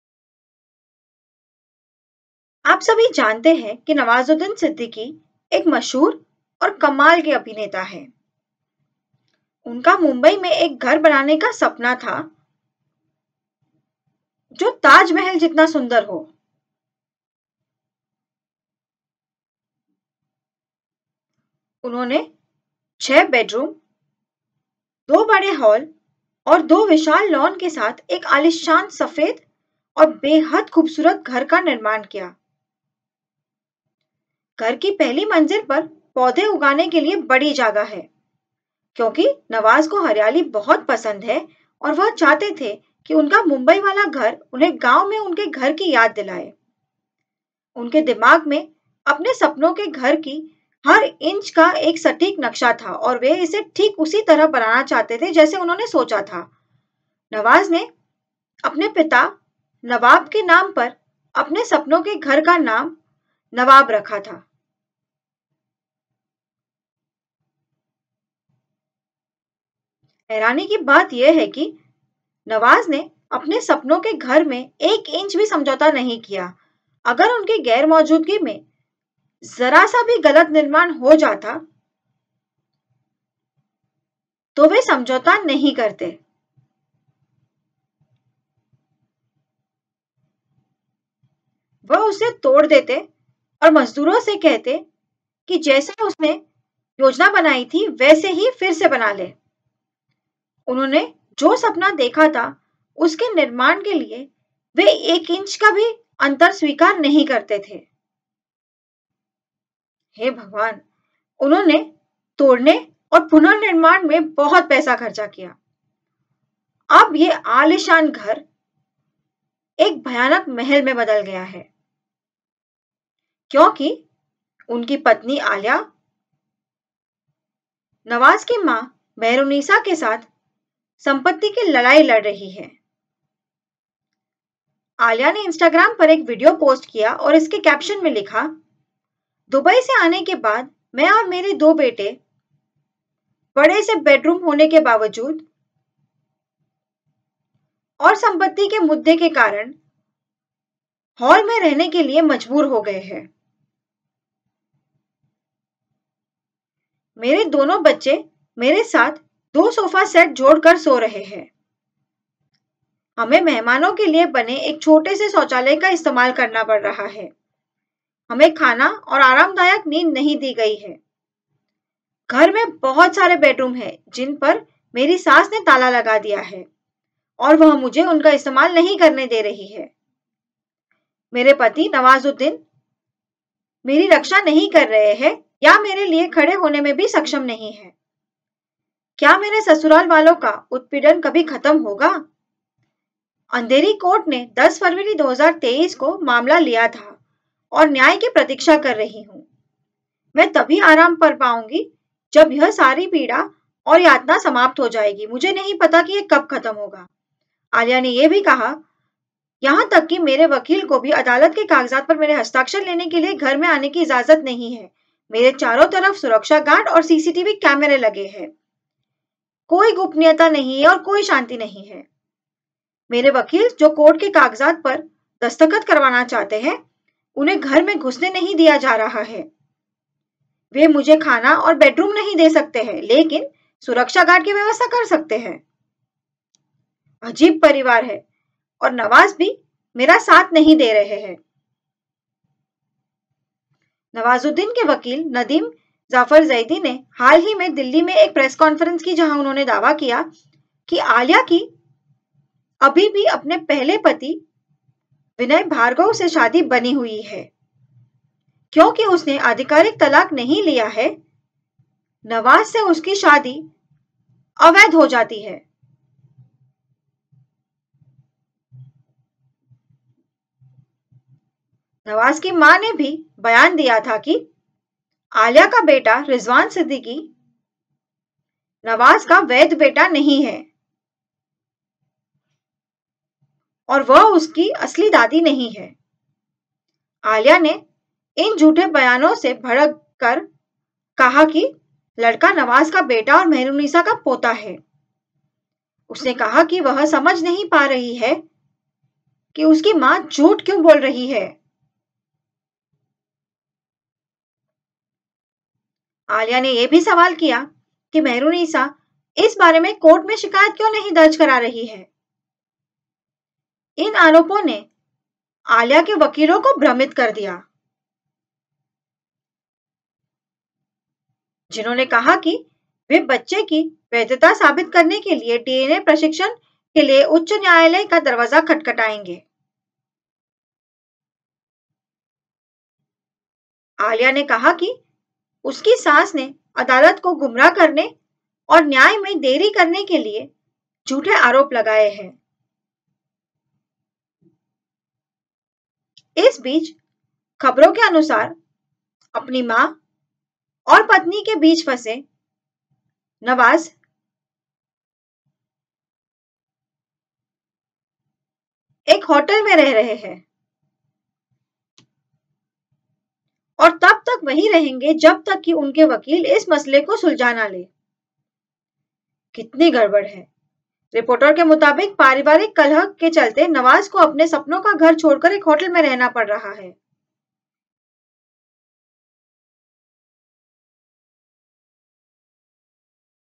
Nawazuddin Siddiqui's Wife Aaliya Forced to Sleep in Hall | ISH News
Brick by brick, Nawazuddin Siddiqui constructed his own Taj Mahal in Mumbai. Gleaming white and custom-built, Nawaz is very proud of his house. Nawaab, Nawaz’s home, named after his father Nawaab, has six bedrooms, two large halls, and two spacious lawns. On the first floor, Nawaz has a large space to grow trees. Nawaz loves greenery. He wants his home in Mumbai to remind me of my home in my village.  His voice beaming with pride and joy, Nawaz said, “I had an exact map of every inch of my dream house in my head, and I would not compromise on even an inch of that vision. If something was built wrongly during my absence, I broke it. There were many demolished walls before the house happened. I wanted every inch of the house to be the way I had designed it in my mind.”

And now, the same home has turned into a veritable horror castle, where his wife Aaliya is fighting an ugly property battle with Nawaz’s mother Mehrunisa. She posted a video on Instagram and captioned it saying, ‘I have been forced to live, sleep and utilize only the hall, at my own husband's house, for the past 7 days. My children who have just arrived from Dubai are sleeping with me by joining two sofa sets in the Hall. I somehow managed to take a bath in a small toilet meant for guests. No food, no sleep and on top of that Security Guards are put all around me. Now, even cameras are installed and every move is monitored. No peace and no privacy. All seven bedrooms are locked by my in-laws, and my husband Nawazuddin Siddiqui is not able to protect me or even stand up for me.  Even my Advocate was not allowed to take my signature for Court papers. Will the harassment by my in-laws ever end. Andheri court took up the matter on 10th February 2023. Waiting for justice.”

Nawazzudin Siddiqui’s lawyerNadeem Zafar Zaidi recently held a press conference in Delhi where they claimed Aaliya is still married to her first husband, Vinay Bhargav. Nawaz’s mother also made a statement saying that their son Rizwan Siddiqui is not the legitimate son of Nawaz.

Aaliya was very angry by these statements.  Aaliya’s lawyer has hinted at approaching the high court for DNA testing to prove the child’s legitimacy and said such baseless allegations are to mislead the court, if they have proof they should have appeared in court and filed their reply to court.

Meanwhile, according to reports, stuck between his mother and wife, Nawaz has moved into a hotel and will remain there until his lawyers sort out the mess. Nawaz had to leave his dream house and stay in a hotel due to the family feud.